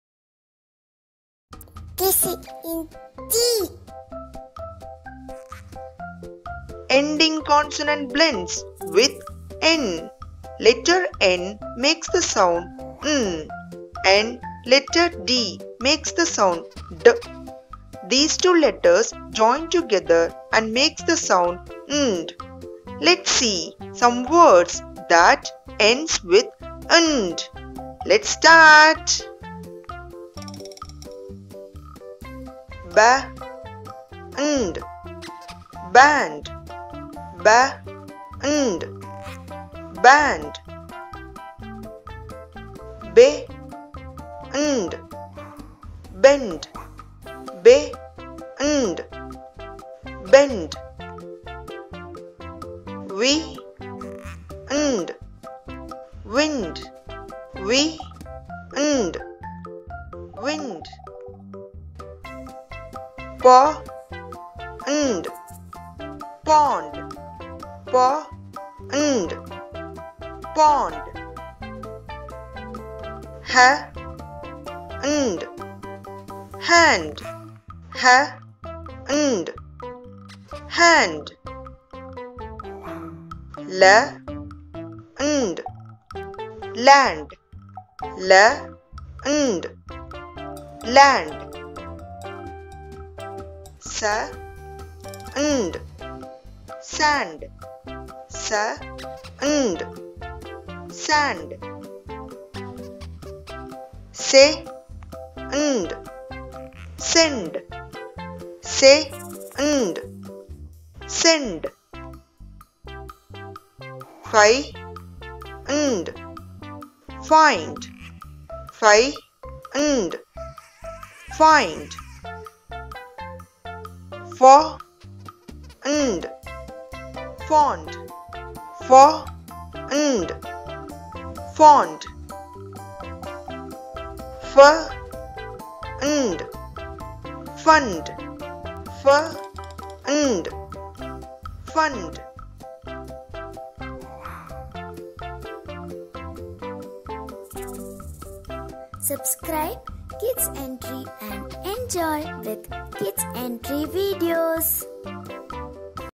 Ending Consonant Blends nd for UKG | Phonic Blends |n blends |nd words -Kids Entry
Ending consonant blends nd

sand
land
hand
fund
fond
pond
wind
bend
find
send
band

Ending Consonant Blends nk for UKG

Ending Consonant Blends nt for UKG

Ending Consonant Blends sp for UKG

Ending Consonant Blends st for UKG

Ending Consonant Blends sk for UKG